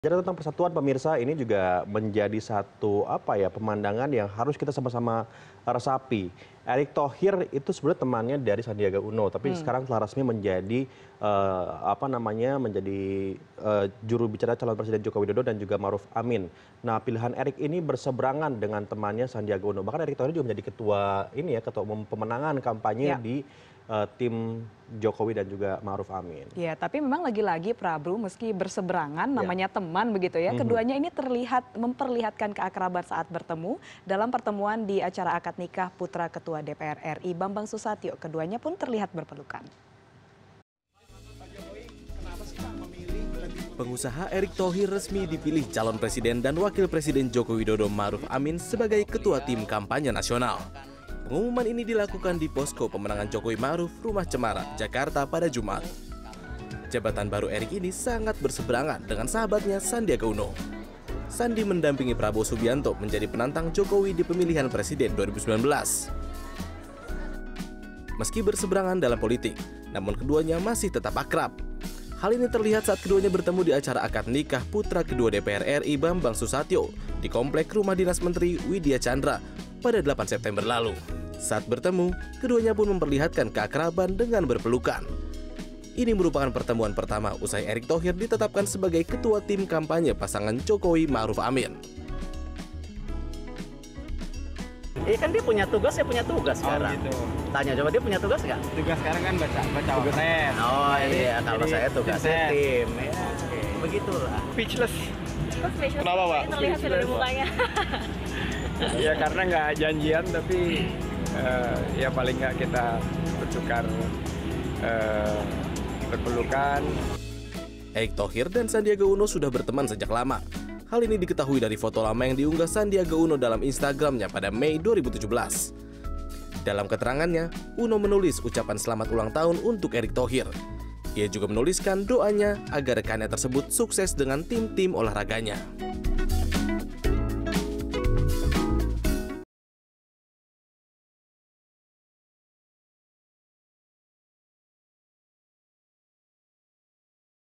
Sahabatan Sejak Lama, Sandiaga & Erick Thohir Berpelukan
Erick Thohir telah resmi menjadi ketua pemenangan calon Presiden Joko Widodo dan Ma'ruf Amin. Pilihan Erick ini berseberangan dengan temannya, Sandiaga Uno. Meski berseberangan, keduanya masih memperlihatkan keakraban saat bertemu. Dalam petemuan di acara akad nikah putra ketua DPR RI, Bambang Soesatyo, keduanya terlihat berpelukan.

Like & Follow Facebook:
CNN Indonesia

cnnindonesiatv